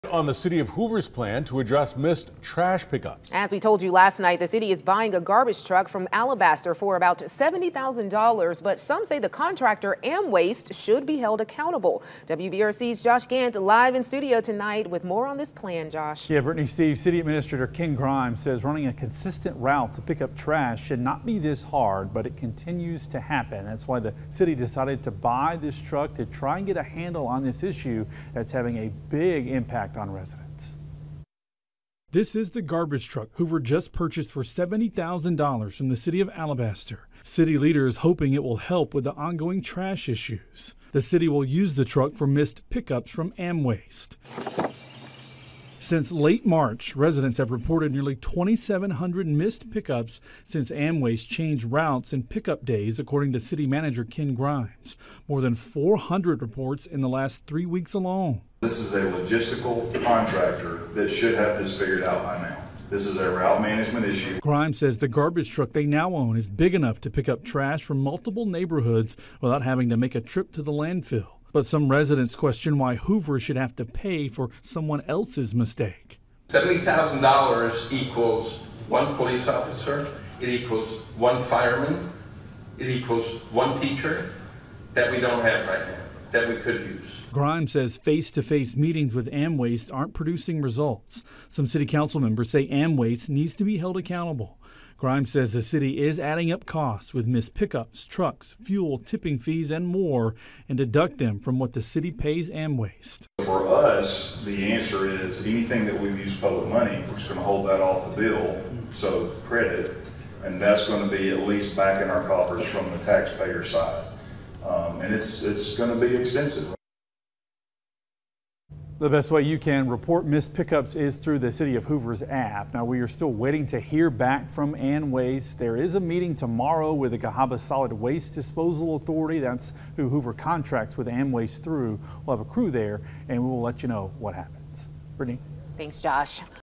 Hoover buying trash truck from Alabaster
For more stories like these, visit WBRC.com